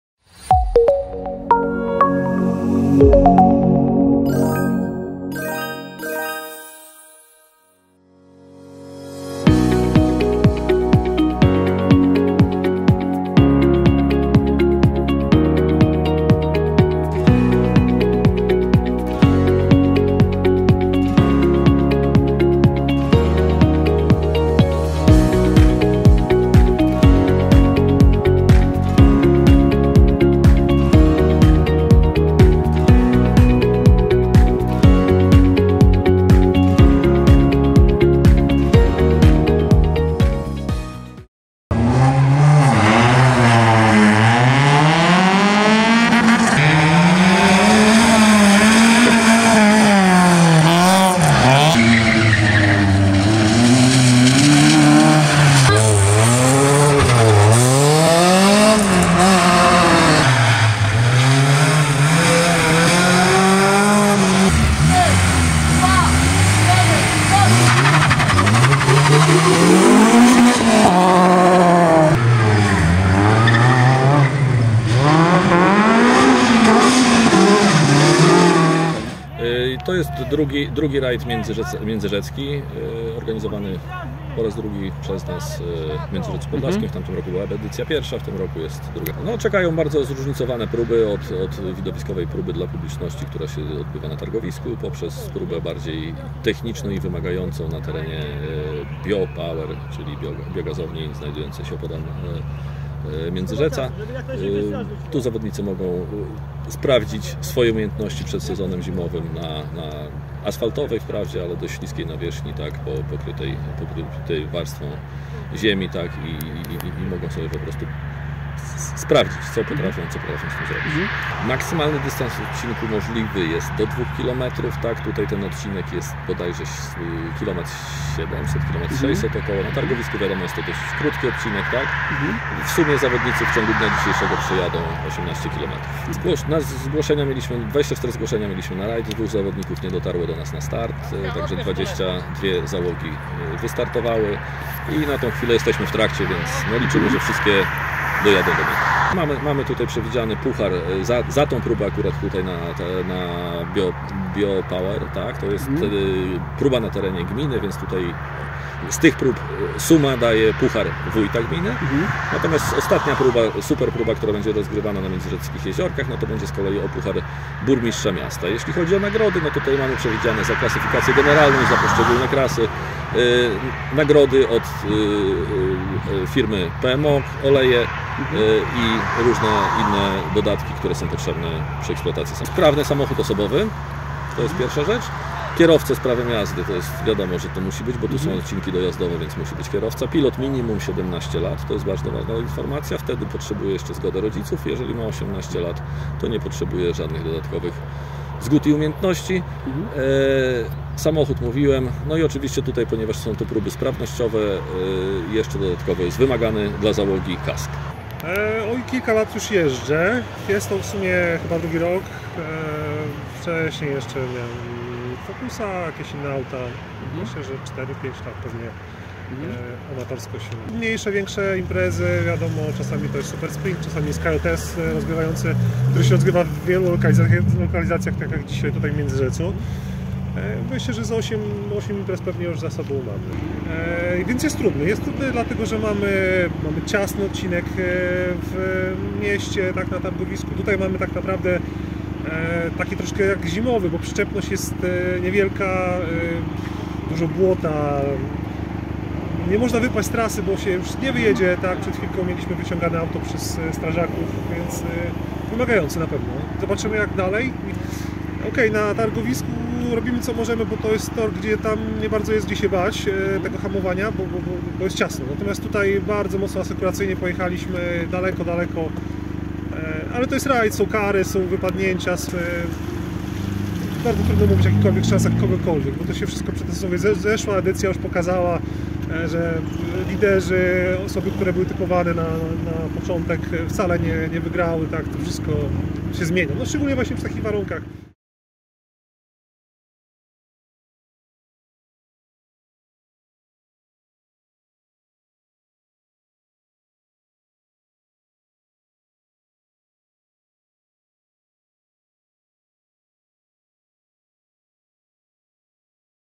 II Rajd Międzyrzecki| Radio Biper
2. Rajd Międzyrzecki 2. Runda Samochodowych Mistrzostw Automobilklubu Bialskopodlskiego 1. Runda Mistrzostw Okręgu Lubelskiego. Impreza odbyła się 19 listopada na terenie miasta i gminy Międzyrzec Podlaski.